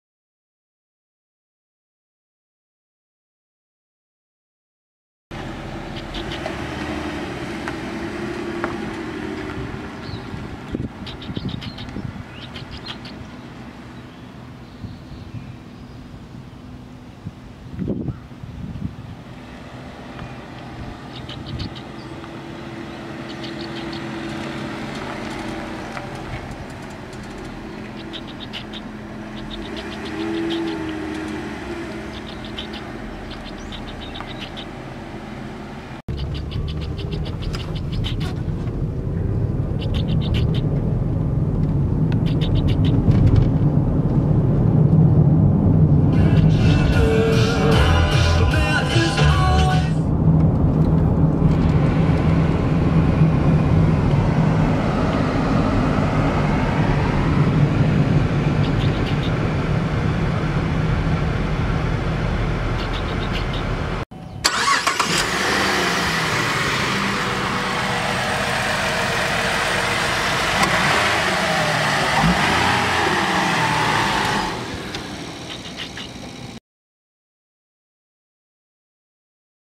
2008 GMC Acadia SLT Online at Tays Realty & Auction, LLC
Absolute Online Auction: 2008 GMC Acadia & 2002 Subaru Impreza

Selling June 26 through July 10, 2018